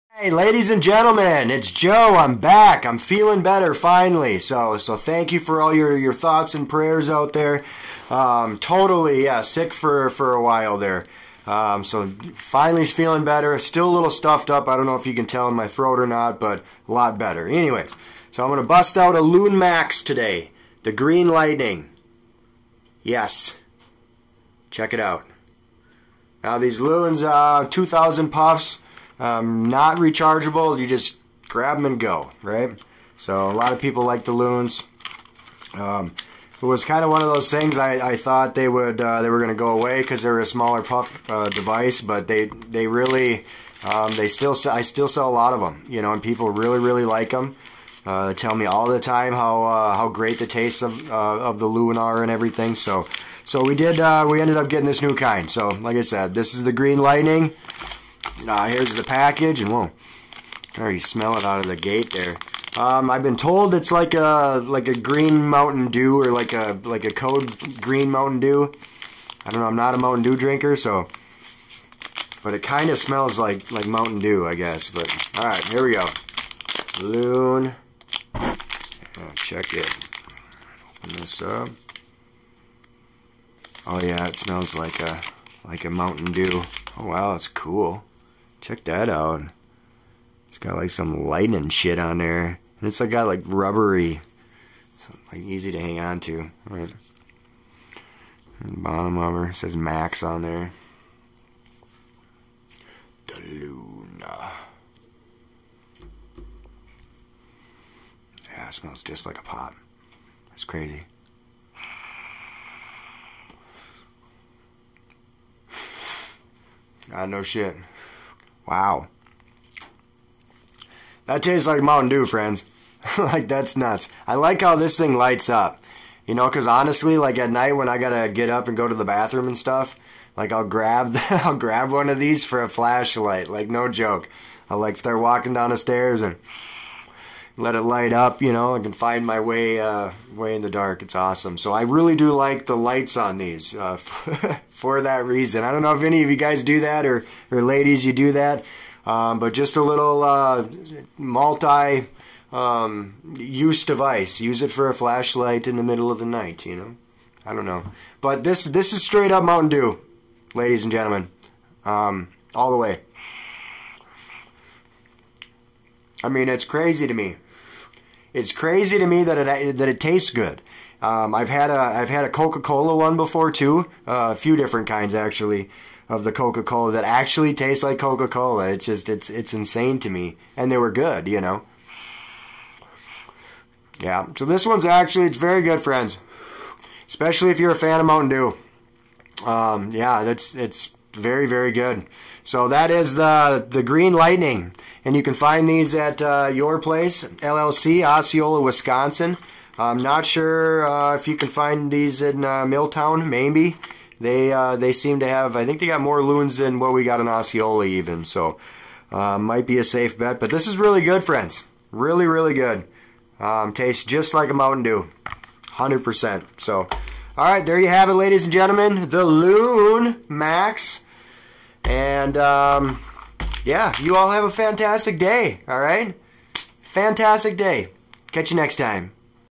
Loon Maxx! 💨 Green Lightning💨 Small Device, Big Flavor!💨
Loon Maxx... Small Device, Big Taste! "Green Lightning"
Review by Your Place LLC with Joe

2000 Puffs

Grab and go!